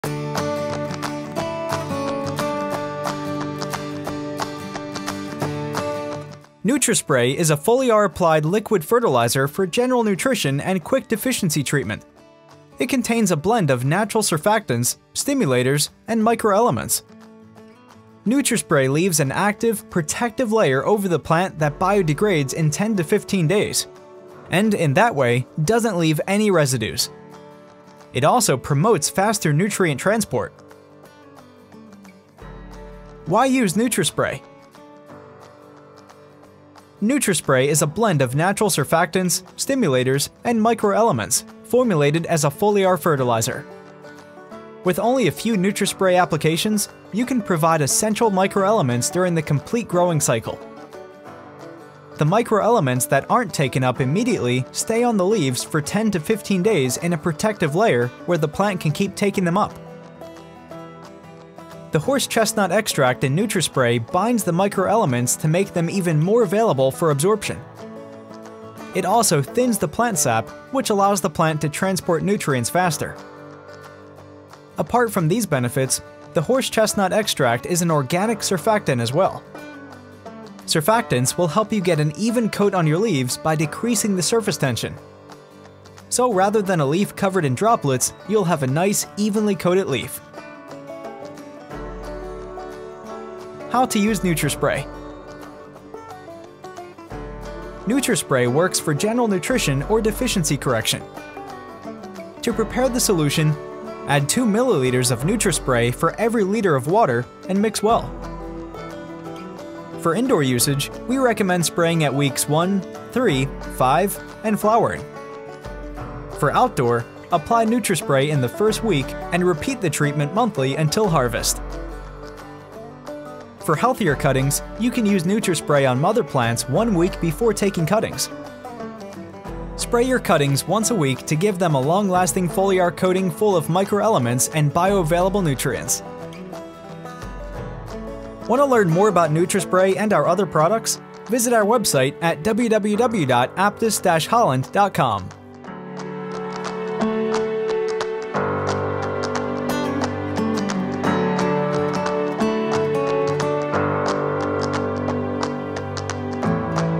NutriSpray | Aptus Plant Tech | Foliar Applied Liquid Fertilizer
NutriSpray is a blend of natural surfactants, stimulators and microelements, to be used as a foliar fertilizer. It provides essential microelements during the complete growing cycle and leaves a natural coating on the leaf to protect and to feed. NutriSpray can be used for general nutrition or for deficiency correction.